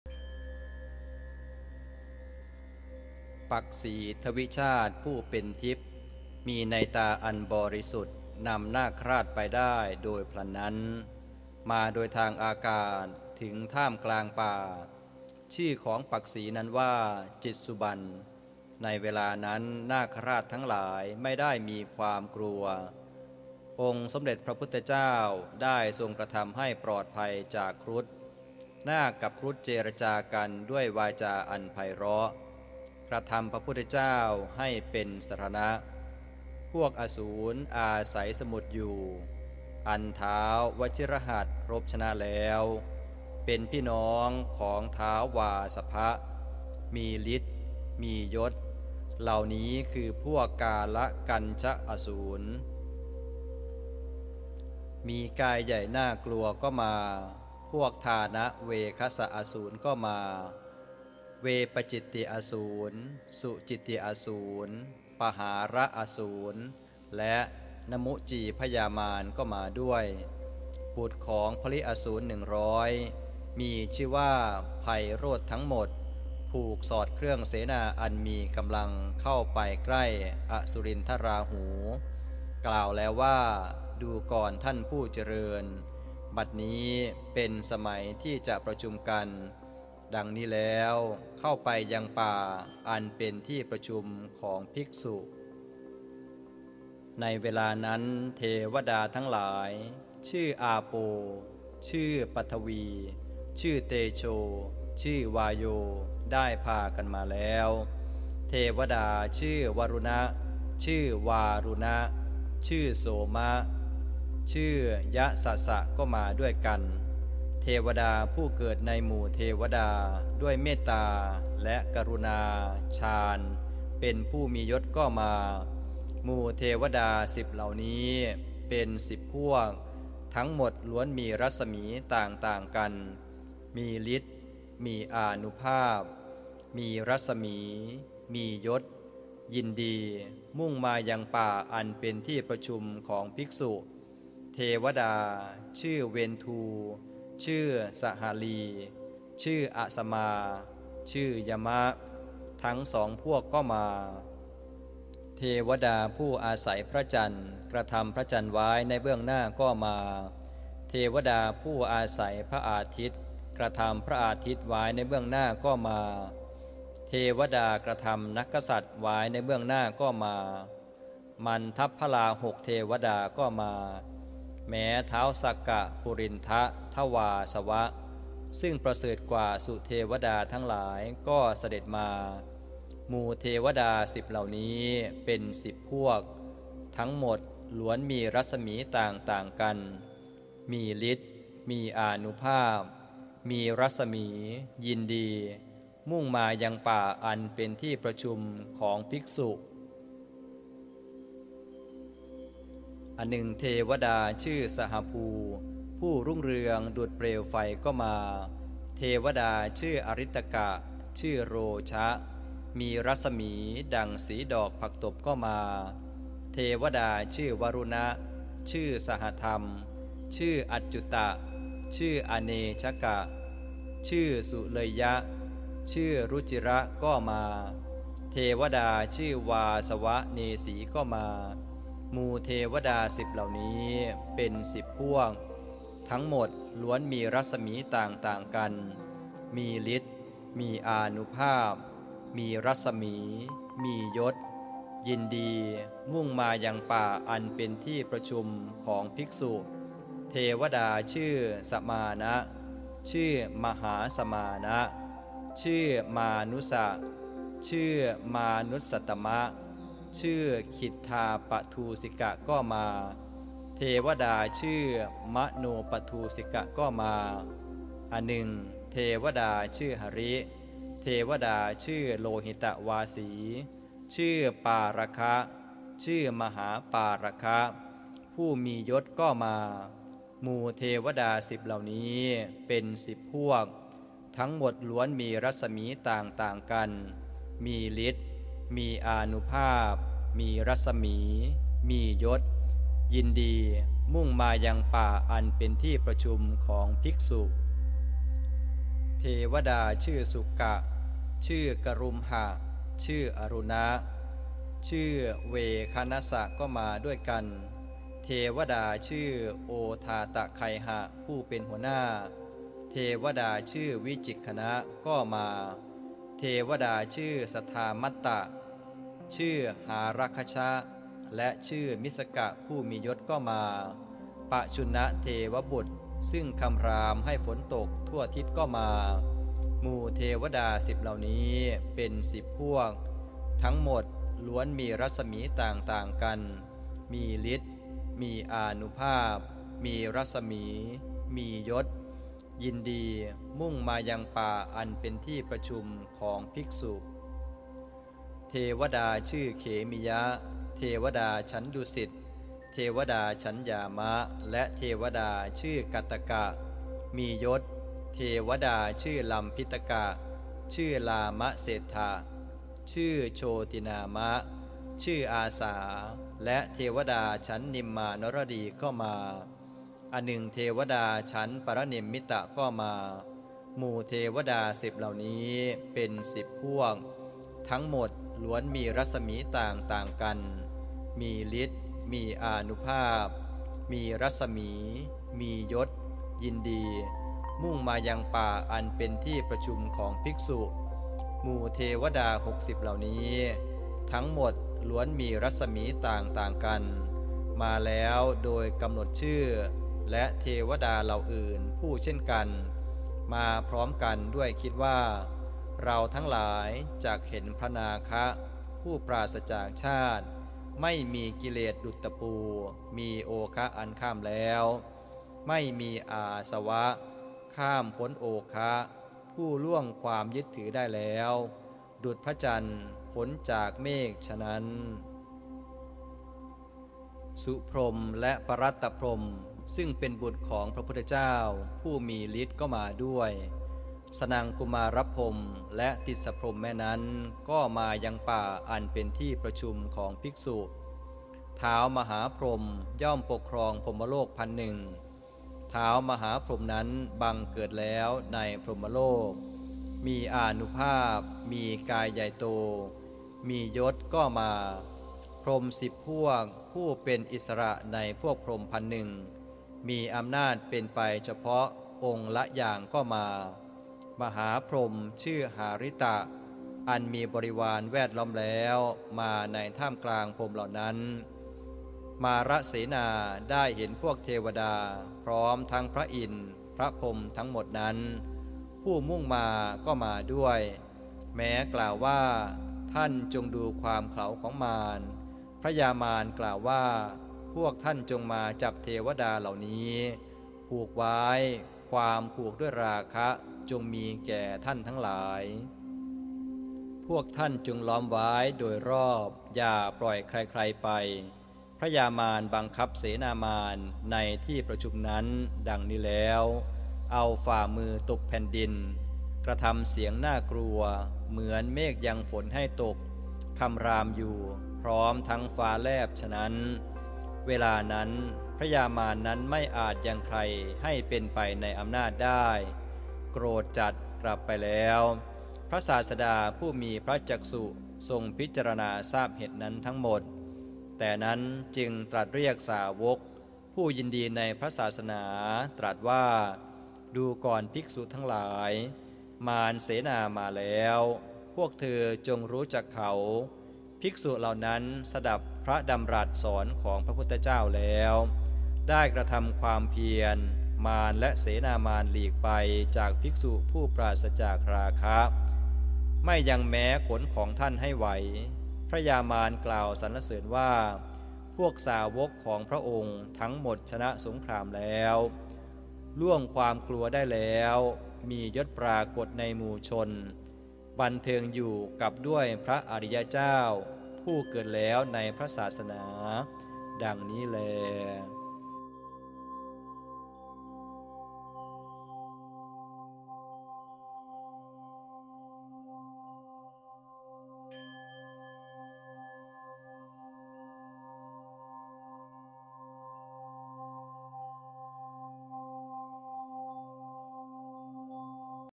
มหาสมัยสูตร ประชุมใหญ่ของเหล่าเทวดา ยักษ์ นาค คนธรรพ์ ตอนจบ
มหาสมัยสูตร ประชุมใหญ่ของเหล่าเทวดา ยักษ์ นาค คนธรรพ์ พรหม ใน 10 โลกธาตุมาประชุมกันเพื่อเข้าเฝ้าพระพุทธเจ้า ณ ป่ามหาวัน เขตกรุงกบิลพุสดุ์